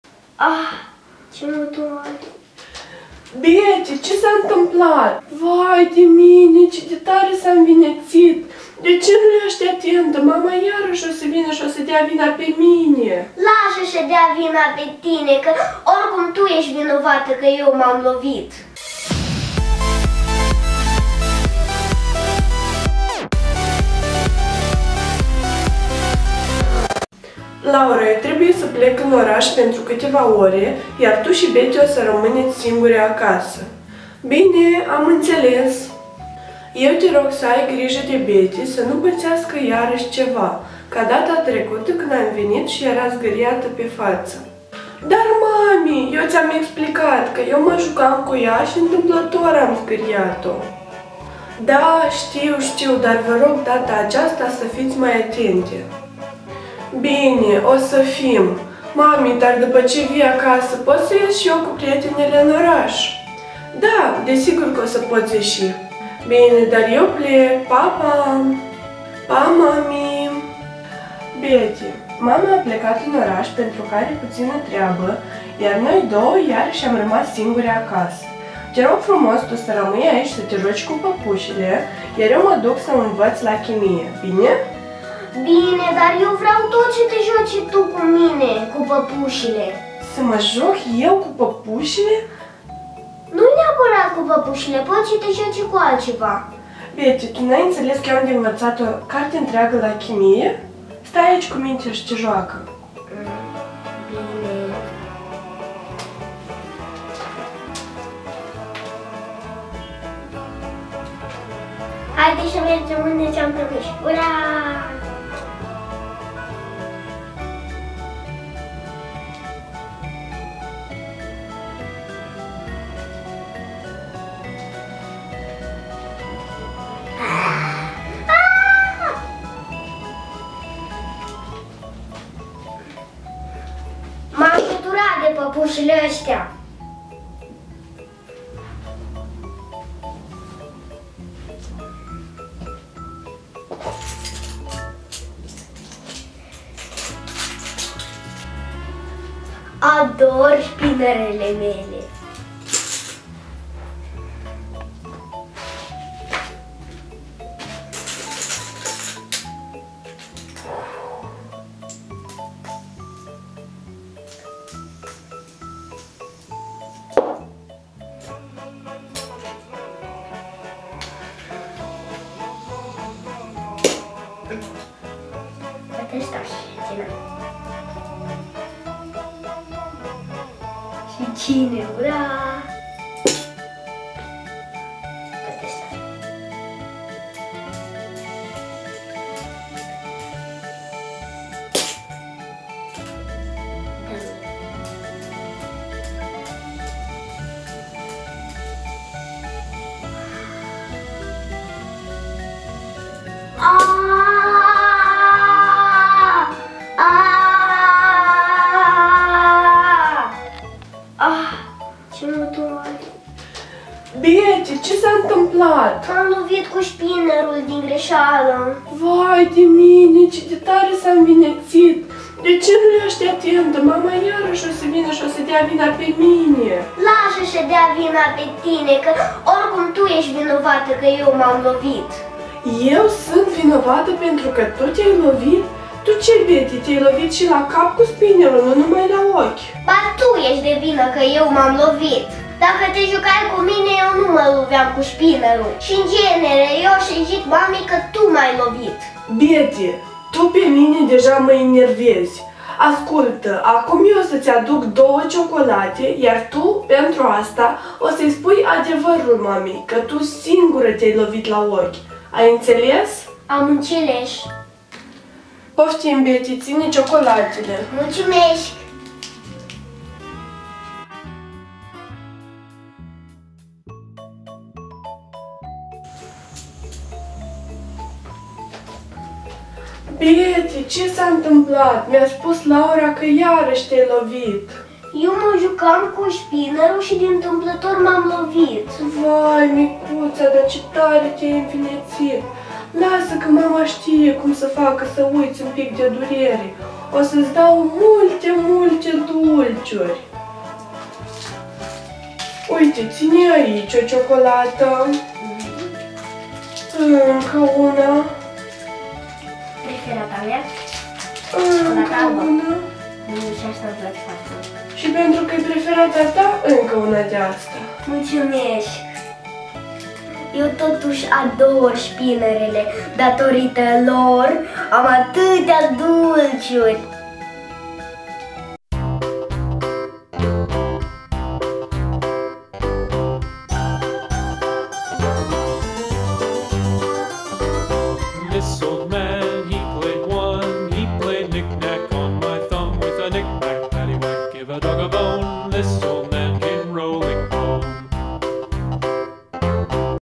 Betty s-a lovit cu SPINNERUL la ochi Poveste pentru copii
Mai multe Povesti pentru copii cu Laura si Betty puteti vedea aici

In aceste desene animate pentru copii Betty se loveste la ochi cu fidget spinerul.